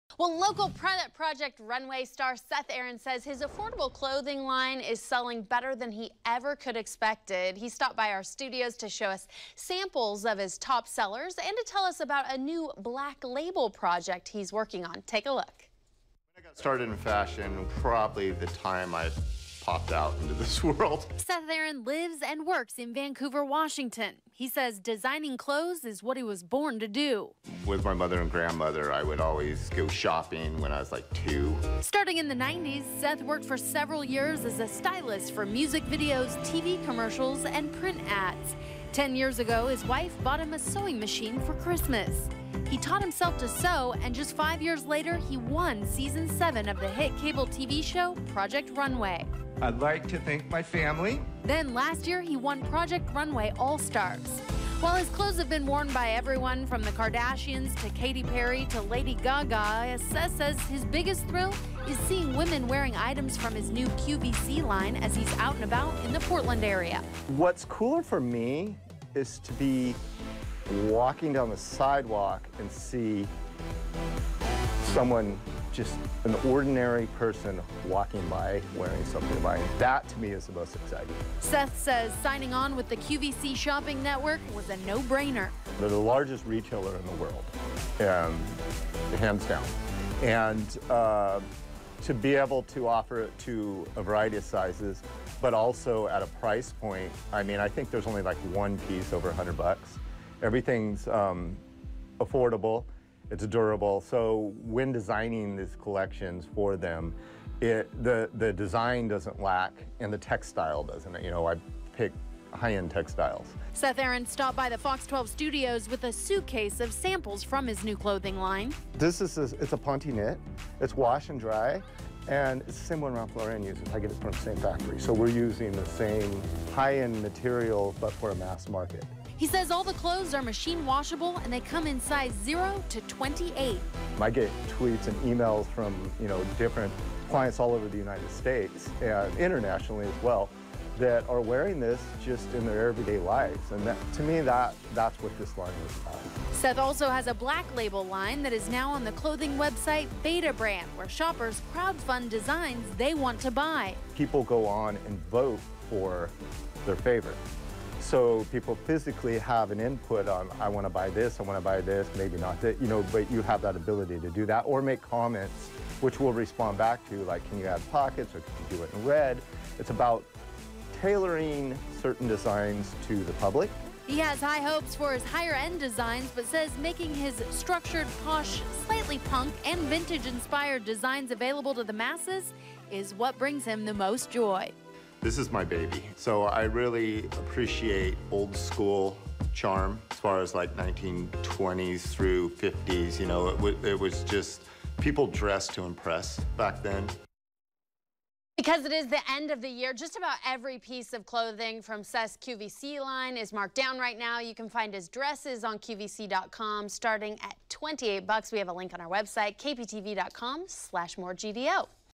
'Project Runway' winner Seth Aaron's new designs
Local designer Seth Aaron talks to More about his two latest design projects and where you can get them for as low as $24.